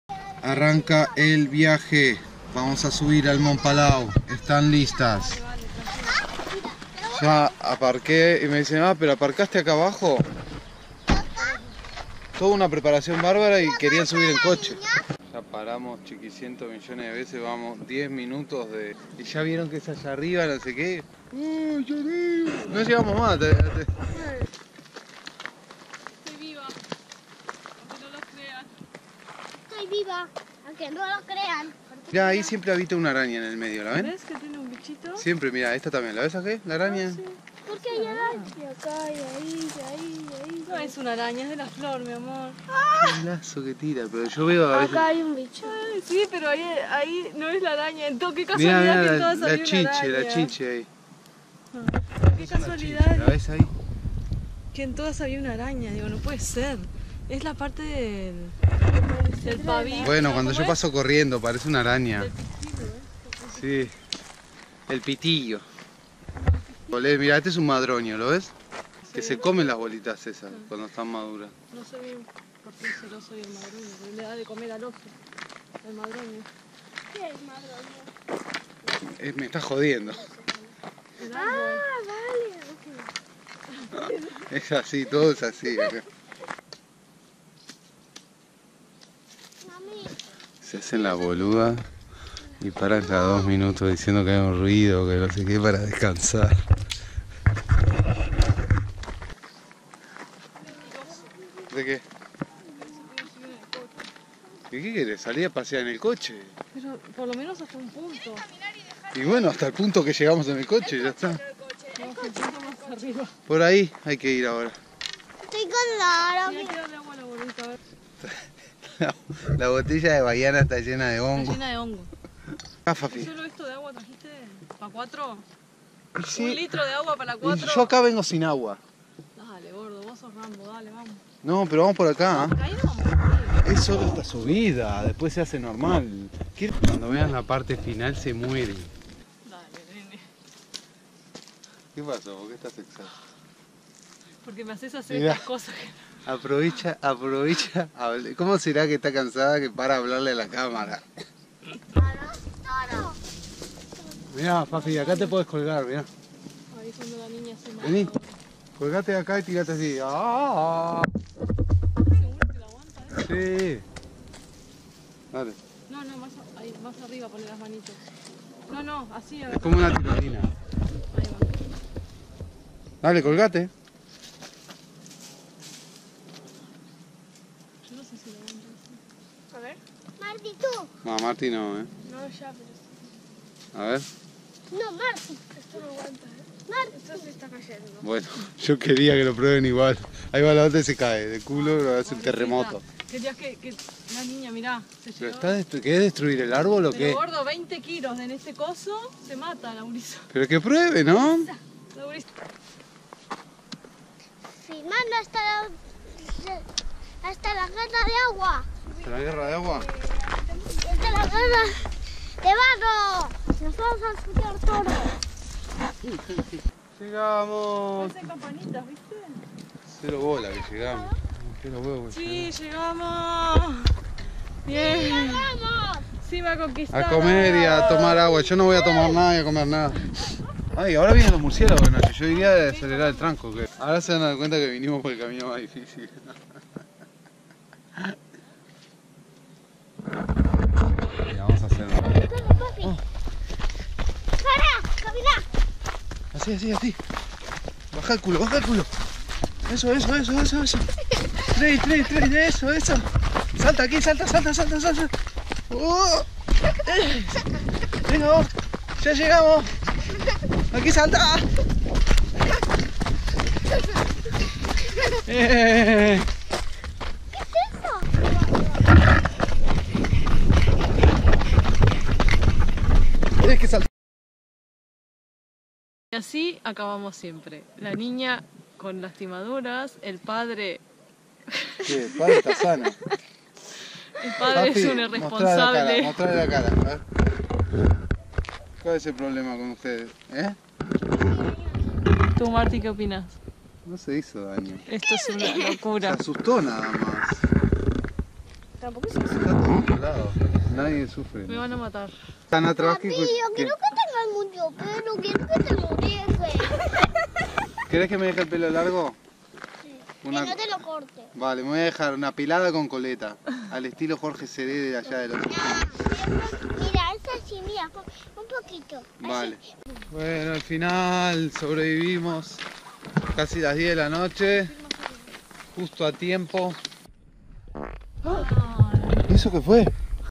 Subida al Montpalau en Familia
Una subida en Familia al Montpalau de Pineda de Mar, poca agua, botellas contaminadas, caídas y sobre todo muchísimas quejas! Así nos gusta a nosotros!